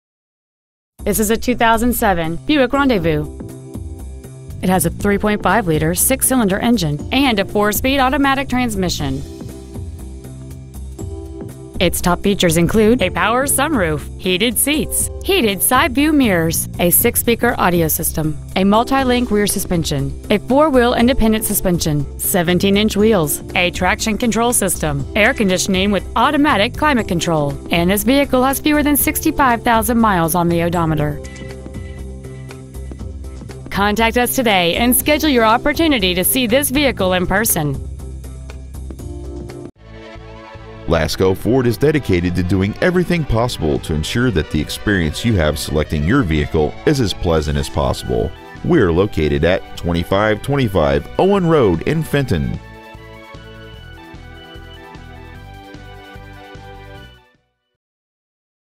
2007 BUICK RENDEZVOUS MI
We are proud to present this 2007 BUICK RENDEZVOUS .

 Year : 2007

 Make : BUICK

 Model : RENDEZVOUS

 Engine : 3.5L V6

 Trans . : AUTO 4SPD

 Exterior : SILVER

 Miles : 64,960

 Interior : GRAY

 Lasco Ford

 2525 Owen Road